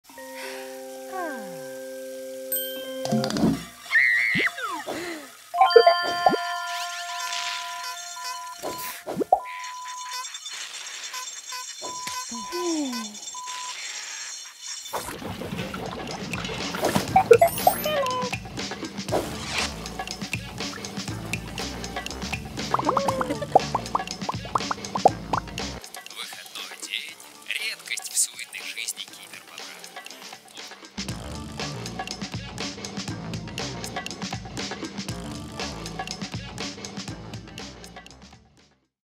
Цифровая безопасность для тинейджеров
КиберБобер знает, как повысить интерес тинейджеров к цифровой безопасности, загляните в его инструкции.